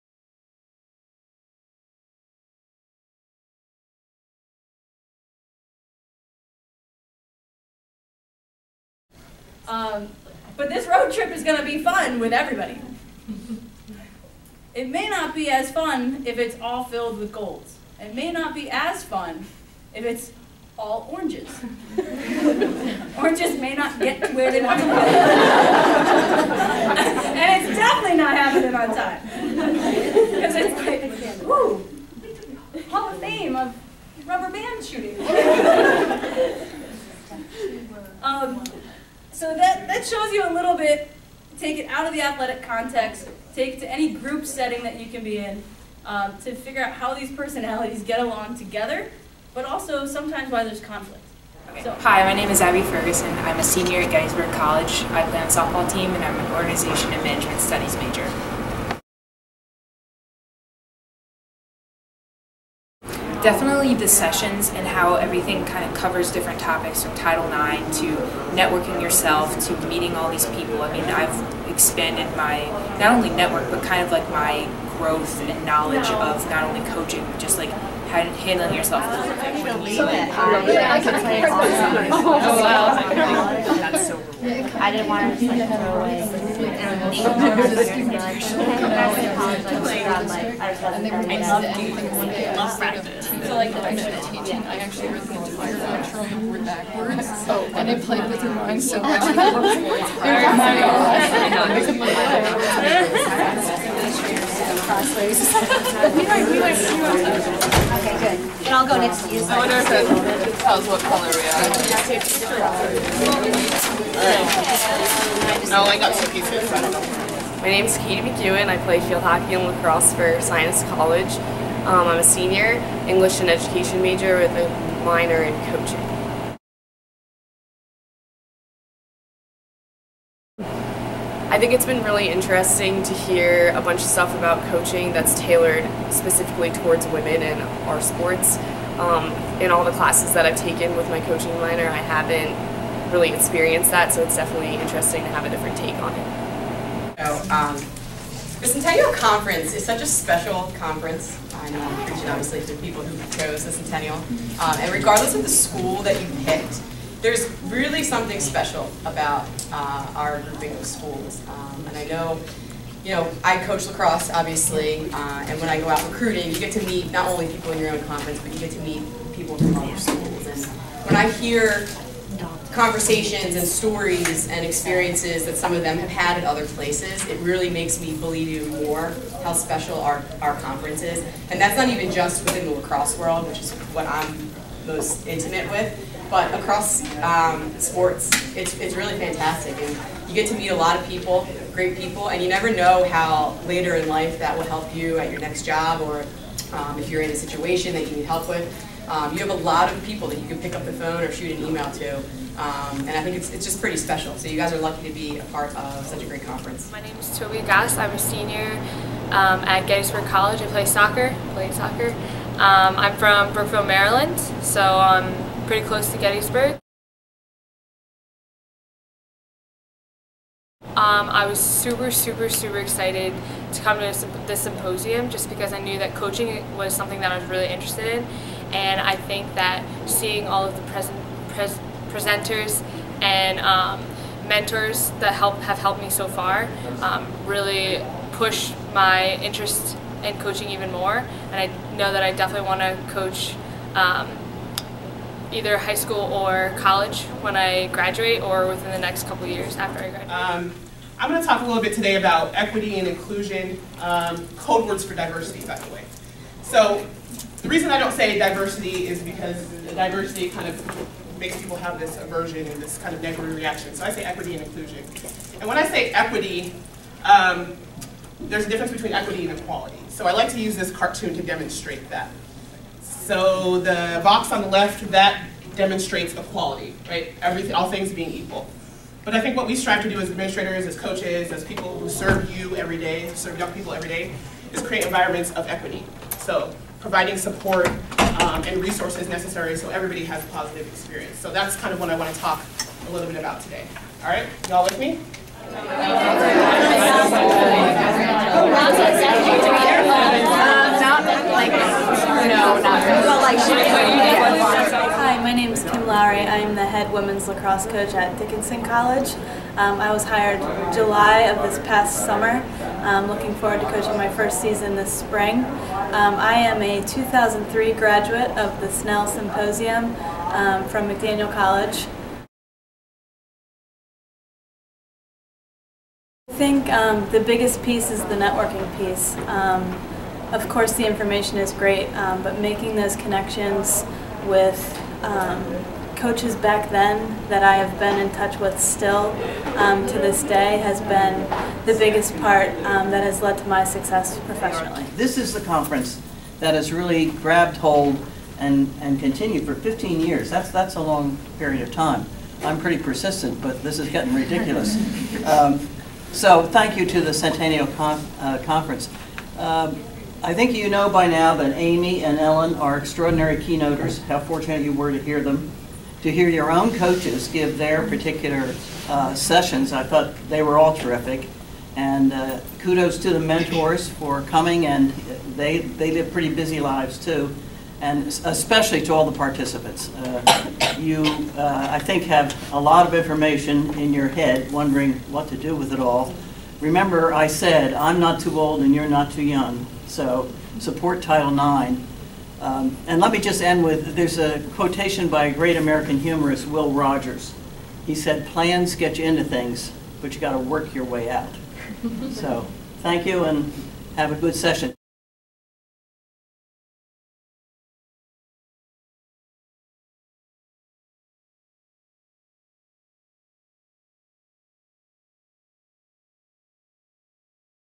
2014 Snell Shillingford Symposium Recap
The 15th annual Snell-Shillingford Coaching Symposium for undergraduate women was held at Ursinus College on January 24-26.